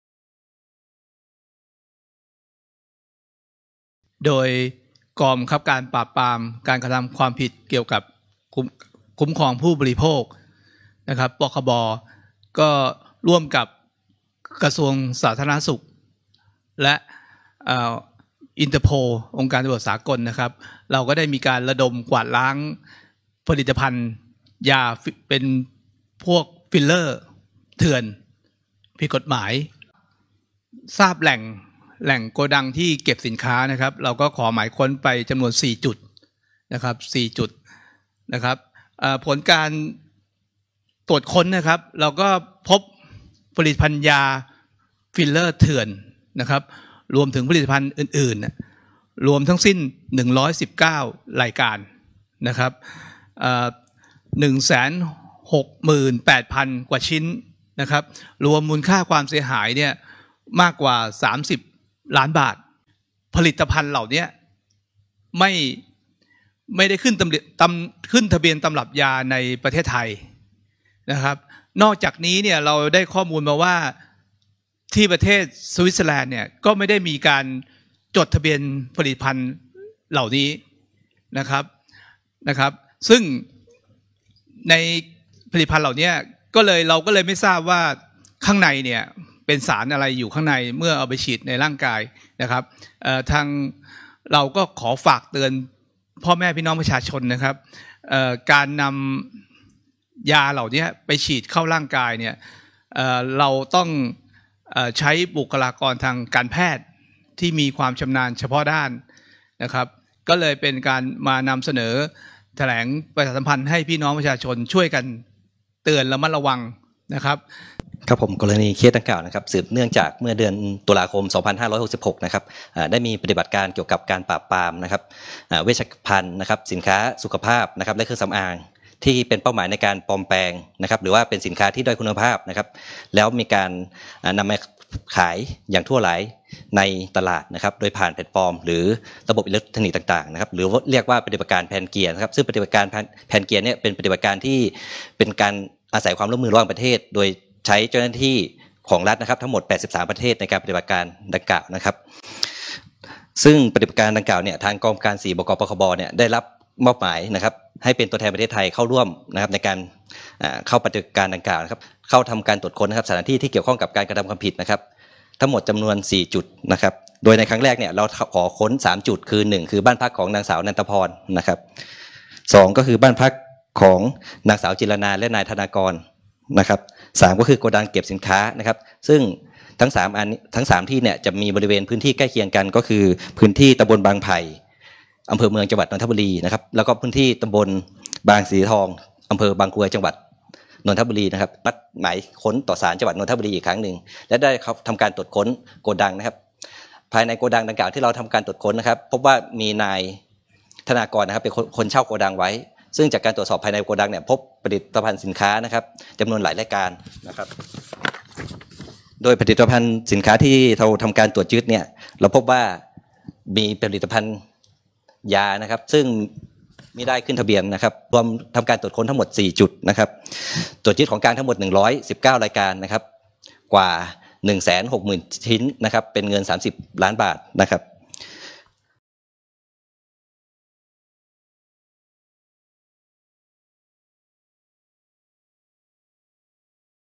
CIB ร่วมกับตำรวจสากล จับกุมเครือข่ายลักลอบจำหน่ายฟิลเลอร์ผิดกฎหมาย ยึดของกลางกว่า 1.6 แสนชิ้น
CIB ร่วมกับตำรวจสากล(INTERPOL) จับกุมเครือข่ายลักลอบจำหน่ายฟิลเลอร์ผิดกฎหมาย ยึดของกลางกว่า 1.6 แสนชิ้น

กองบัญชาการตำรวจสอบสวนกลาง โดย บก.ปคบ., สำนักงานกลางแห่งชาติตำรวจสากลกรุงเทพ (Interpol NCB Bangkok) ร่วมกันแถลงปฏิบัติการจับกุมเครือข่ายลักลอบจำหน่ายผลิตภัณฑ์สุขภาพผิดกฎหมายข้ามชาติ ตรวจค้น 4 จุด ตรวจยึดผลิตภัณฑ์ยา ผลิตภัณฑ์เครื่องมือแพทย์ ผลิตภัณฑ์เครื่องสำอาง รวม 119 รายการ รวมทั้งสิ้น 168,781 ชิ้น มูลค่ากว่า 30,000,000 บาท

สืบเนื่องจากกองกำกับการ 4 กองบังคับการปราบปรามการกระทำความผิดเกี่ยวกับการคุ้มครองผู้บริโภค ได้รับการประสานงานจาก องค์การตำรวจสากล( INTERPOL) ให้ทำการสืบสวนกลุ่มผู้กระทำความผิด กรณีเมื่อประมาณ เดือนตุลาคม 2566 ประเทศสิงคโปร์ได้ตรวจยึดพัสดุไปรษณีย์บรรจุสินค้าผิดกฎหมาย ซึ่งผู้บริโภคในประเทศสิงคโปร์สั่งซื้อทางแอพพลิเคชั่นไลน์ เป็นผลิตภัณฑ์เสริมความงาม ยี่ห้อ “PLACENTA” เมื่อองค์การตำรวจสากลได้ตรวจสอบการจดทะเบียนของผู้ผลิตและการขึ้นทะเบียนผลิตภัณฑ์ ในประเทศสวิตเซอร์แลนด์ ปรากฎว่า ไม่พบข้อมูลการการจดทะเบียนของผู้ผลิตและผลิตภัณฑ์ดังกล่าวในประเทศสวิตเซอร์แลนด์ ตามที่ผู้ขายกล่าวอ้างแต่อย่างใด

เจ้าหน้าที่ตำรวจ กก.4 บก.ปคบ. จึงได้ทำการสืบสวนจนทราบแหล่งที่เก็บผลิตภัณฑ์ และกลุ่มผู้กระทำความผิด ซึ่งจากการตรวจสอบกลุ่มเครือข่ายดังกล่าว ไม่ได้รับอนุญาตให้จำหน่ายยาแผนปัจจุบันแต่อย่างใด ในวันที่ 15 กุมภาพันธธ์ 2567 จึงได้รวบรวมพยานหลักฐานเพื่อขออนุมัติหมายค้นต่อศาล จ.นนทบุรี เข้าตรวจค้นทั้งหมด 4 จุด ในพื้นที่ จ.นนทบุรี

ผลการตรวจค้น ตรวจยึดผลิตภัณฑ์ผิดกฎหมาย ยี่ห้อเดียวกันกับที่ประเทศสิงค์โปร์ตรวจพบ ได้แก่

1.ผลิตภัณฑ์ยา ยี่ห้อ PLACENTA จำนวน 2500 กล่อง

2.ผลิตภัณฑ์ยา ยี่ห้อ BIOCELL จำนวน 800 กล่อง

3.ผลิตภัณฑ์ยา ยี่ห้อ COLLAGEN จำนวน 900 กล่อง